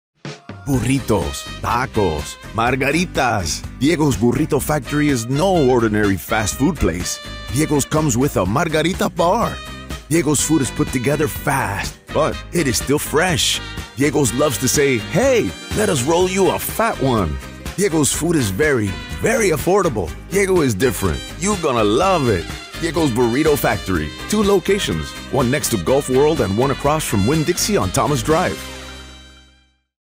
Diego's Burrito Factory
Burritos, tacos, margaritas... Diego's Burrito Factory is no ordinary fast food place!